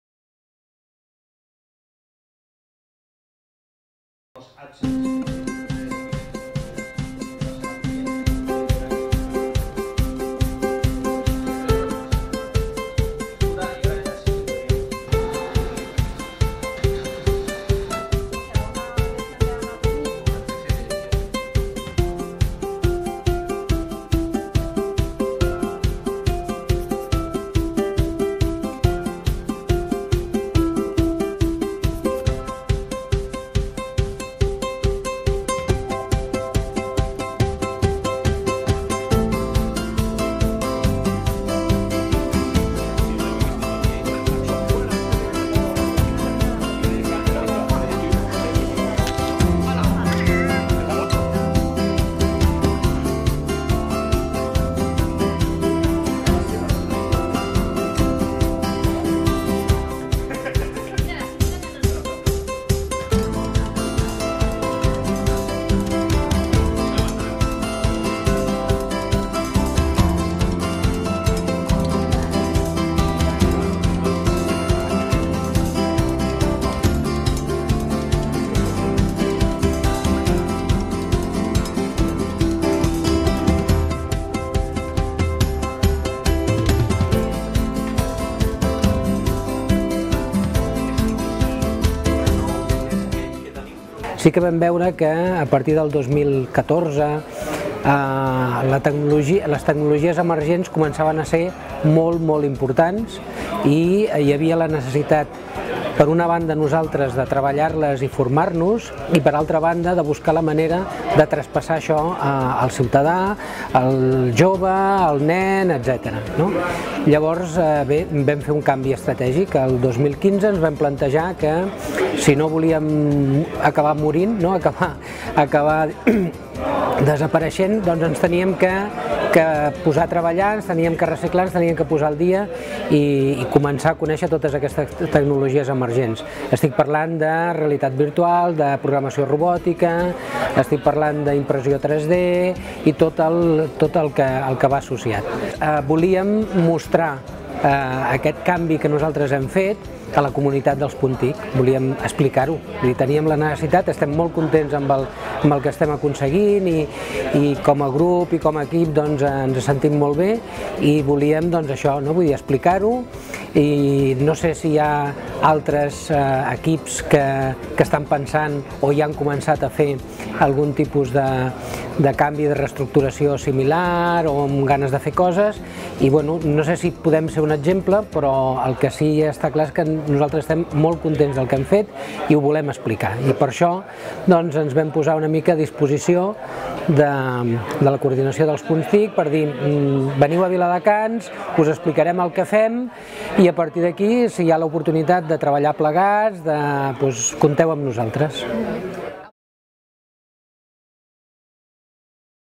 IV Trobada Robotica Creativa
El 24 de març va tenir lloc la IV Trobada Robotica Creativa CodIRob a Viladecans. Es tracta d'un espai per a compartir coneixements i bones pràctiques entre dinamitzadors i dinamitzadores de la Xarxa Punt TIC.